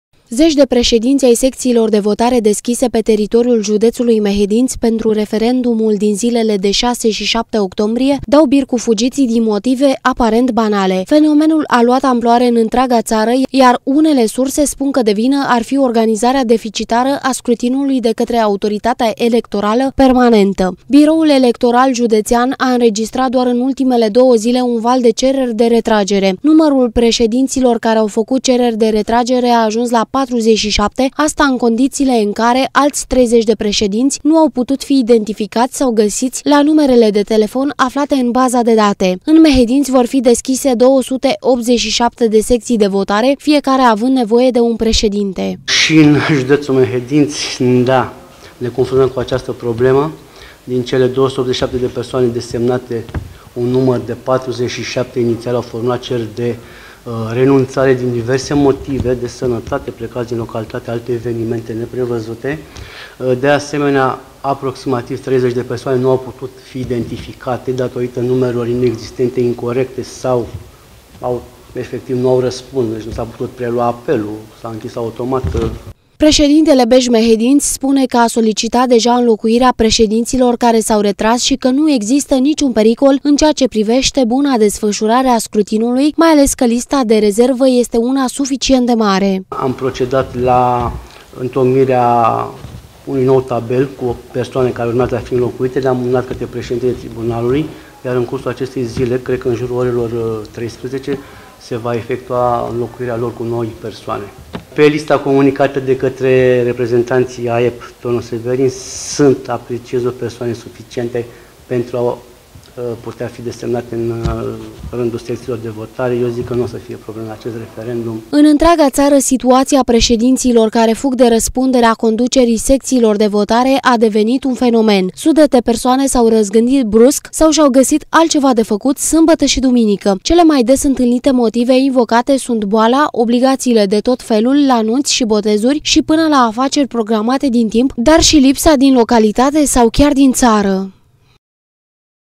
PREȘEDINȚII SECȚIILOR DE VOTARE DAU BIR CU FUGIȚII
Președintele Biroului Electoral Județean Mehedinți, judecătorul Alin Ghimpău, a lămurit, astăzi, situația privind dezertările masive de la conducerea secțiilor de votare. Fenomenul a luat amploare la nivel national, iar Mehedințiul nu face excepție. Zeci de președinți ai secțiilor de votare renunță la acestă calitate, motivele invocate fiind de ordin personal.